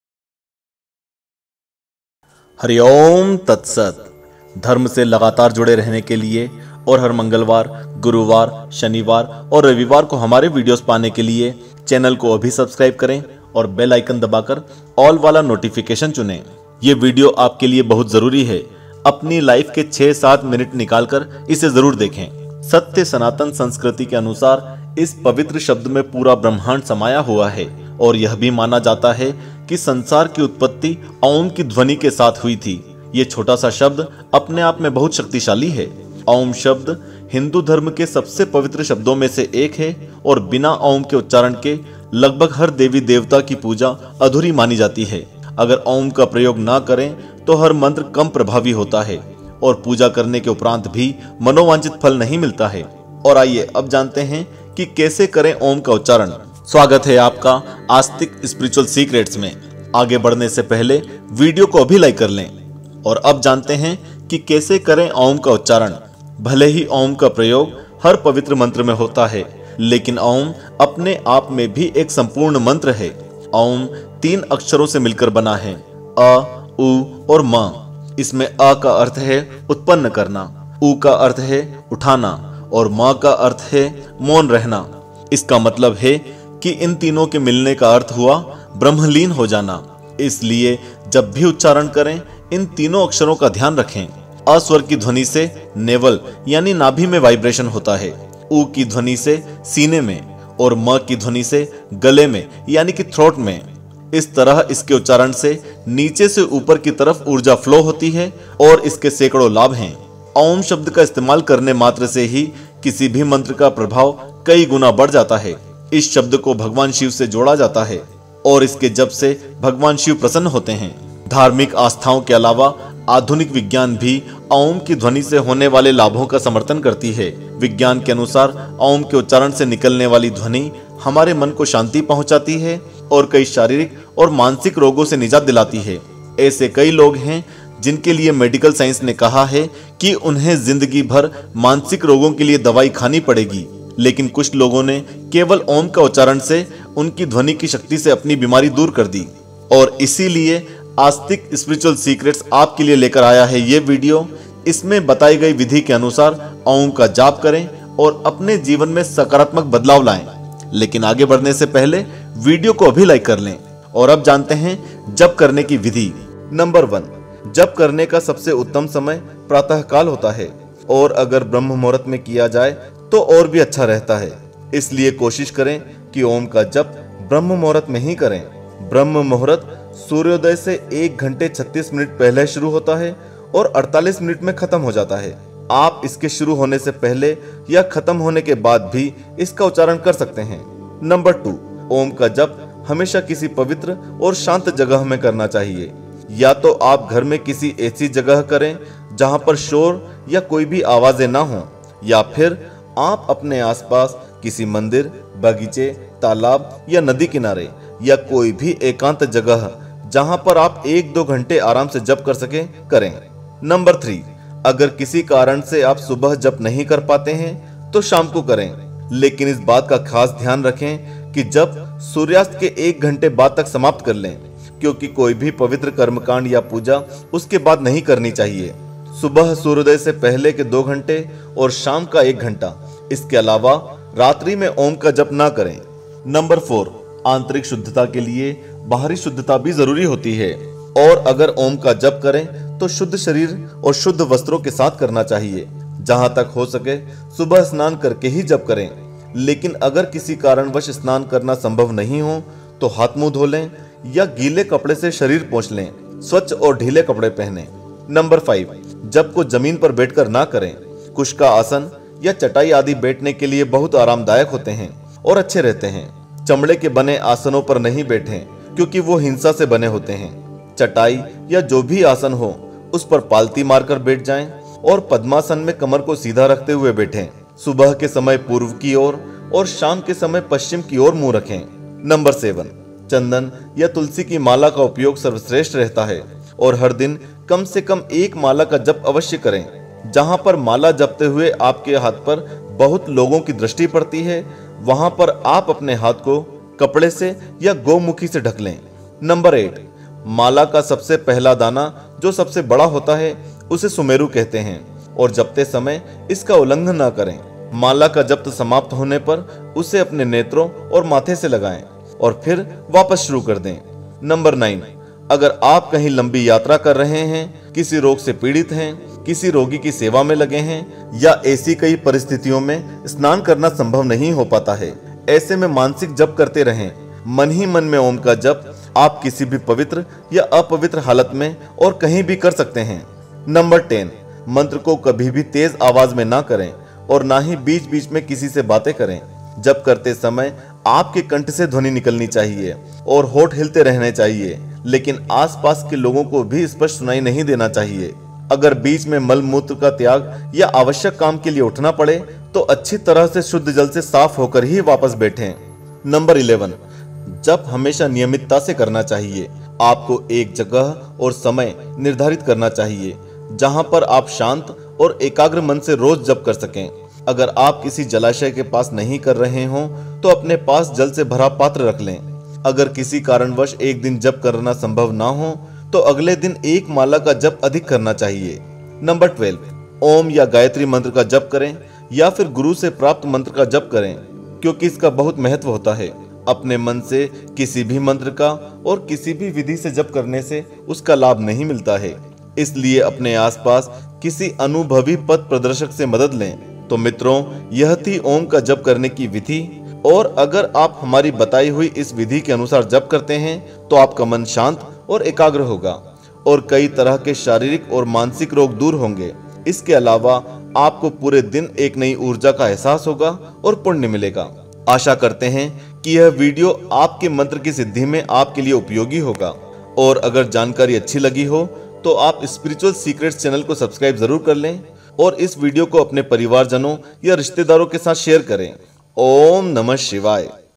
ॐ का उच्चारण इस विधि से करने पर होते है बड़े फायदे | OM Chanting Meditation And Benefits
Today we are going to reveal the OM chanting meditation and benefits. According to Hindu civilization, the whole universe is contained in this sacred word and it is also believed that the world originated with the sound of OM. This small word is very powerful in itself.
The word OM is one of the most sacred words in Hinduism and without the pronunciation of OM, worship of almost every deity is considered incomplete. If you do not use OM, then every mantra is less effective and even after worshiping, you do not get the desired results.
How to pronounce OM - Even though it is used in every holy mantra, but OM is a complete mantra in itself. Consists of three letters, A, U, and M. In this, A means to generate, U means to raise, and M means to remain silent. This means that the meeting of these three means to become Brahmalin. So, whenever you pronounce, keep these three letters in mind. The sound of voice is vibration in the navel, the sound of U in the chest and the sound of M in the throat In this way, the energy flows from the bottom to the top of its pronunciation.
Just by using the word before any mantra, the effect of any mantra increases manifold. This word is associated with Lord Shiva and Lord Shiva is very pleased with its chanting.